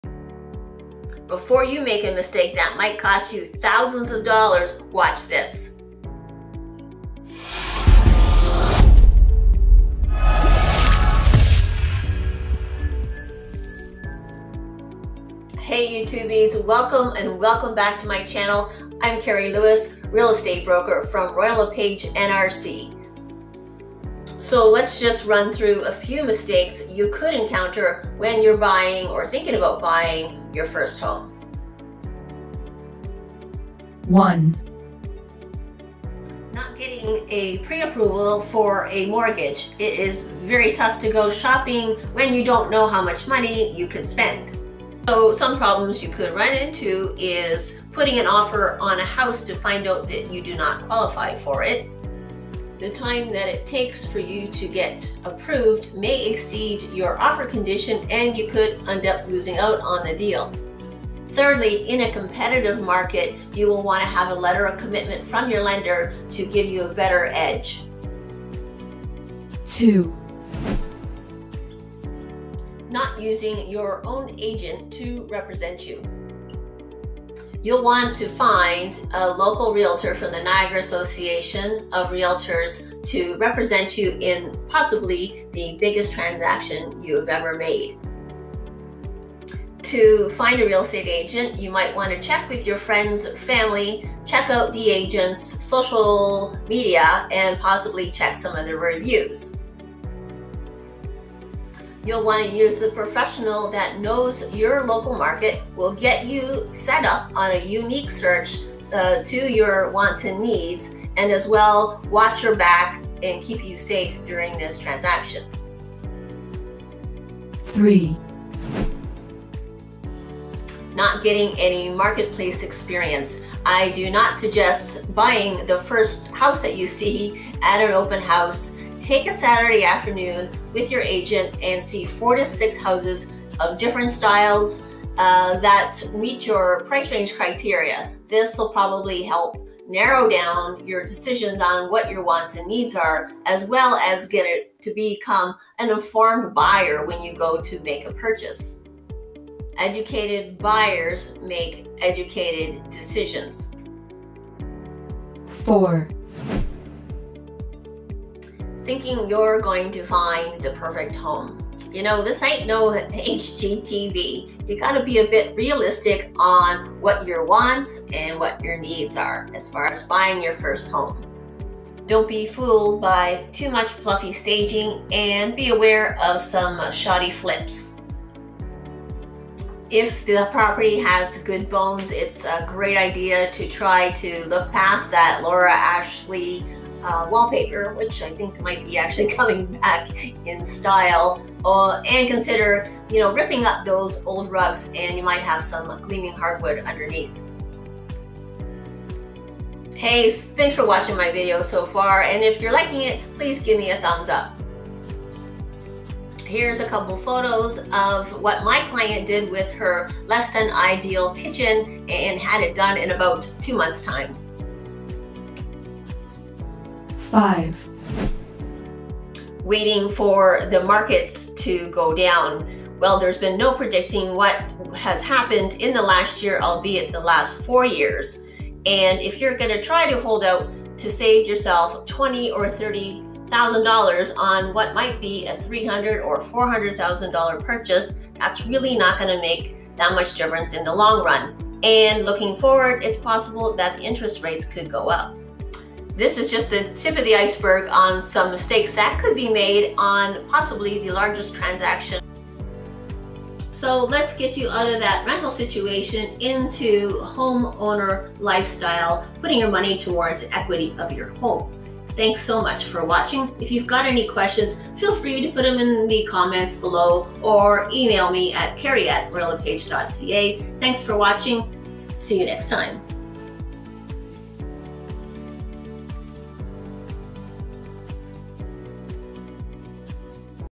1St time homebuyer mistakes
Are you thinking of or ready to buy your 1st home in Niagara? Or maybe this isn't your first purchase but you might get some handy info on your next home purchase. I can offer some mortgage broker suggestions as well.

If you would like a phone or video chat, click here to set up an appointment: